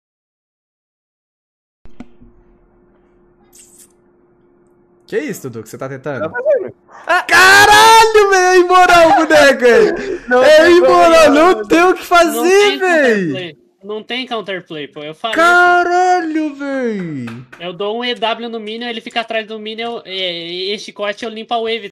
KENNZY ASSISTINDO O X1 DO DUDUDUELISTA CONTRA O QUINDINHO
👑 Quer subir de elo muito mais rápido através de aulas exclusivas com pro-players?
👑 Então acesse e VEM SUBIR DE ELO COM A GENTE!